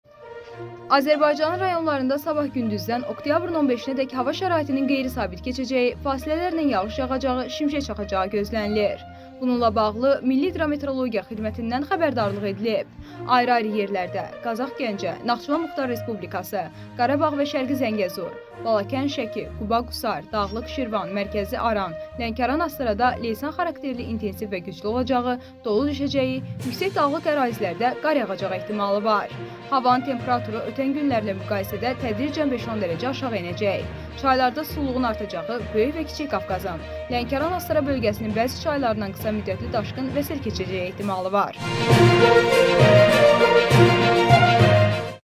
Sabaha gözlənilən hava şəraiti açıqlanıb
► Azərbaycanın rayonlarında sabah gündüzdən oktyabrın 15-dək hava şəraitinin qeyri-sabit keçəcəyi, fasilələrlə yağış yağacağı, şimşək çaxacağı gözlənilir.
Bununla bağlı Milli Hidrometeorologiya Xidməti xəbərdarlıq yayıb.
Ayrı-ayrı yerlərdə: Qazax-Gəncə, Naxçıvan MR, Qarabağ və Şərqi-Zəngəzur, Balakən-Şəki, Quba-Qusar, Dağlıq Şirvan, Mərkəzi Aran, Lənkəran-Astara da leysan xarakterli intensiv və güclü olacağı, dolu düşəcəyi, yüksək dağlıq ərazilərdə qar yağacağı ehtimalı var.
Havanın temperaturu ötən günlərlə müqayisədə tədricən 5-10 dərəcə aşağı enəcək.
Çaylarda sululuğun artacağı, Böyük və Kiçik Qafqazın, Lənkəran-Astara bölgəsinin bəzi çaylarından qısamüddətli daşqın və sel keçəcəyi ehtimalı var.